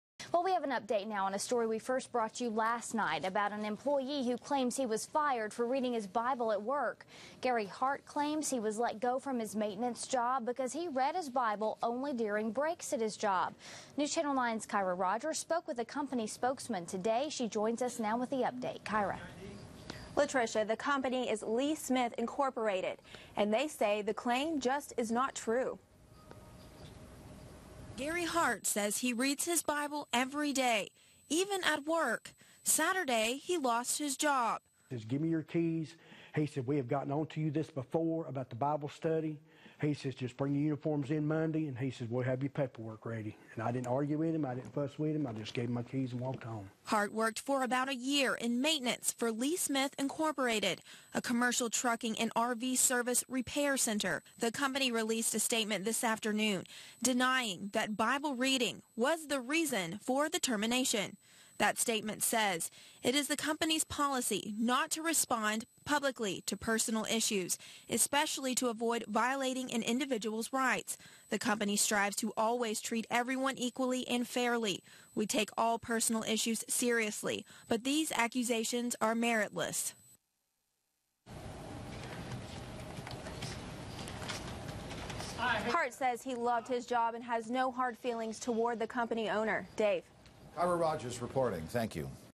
Company Responds to Alleged 'Bible Firing'
by Kyra Rogers
Gary Heart reads his bible every day. He says that is exactly what got him fired from his job on Saturday.
He says give me your keys. We've gotten on to you before about the bible study.He says just bring your uniform in Monday. He says we will have your paperwork ready. I didn't argue or fuss I gave him my keys.
Hart worked for about a year in maintanance for Lee Smith incorporated, a commercial trucking and RV service repair center.�  The company released a statement this afternoon denying that Bible reading was not the reason for the termination.
It is the company's policy not to respond publicly to personnel issues, especially to avoid violating an individual's rights.�  The company strives to always treat everyone equally and fairly.�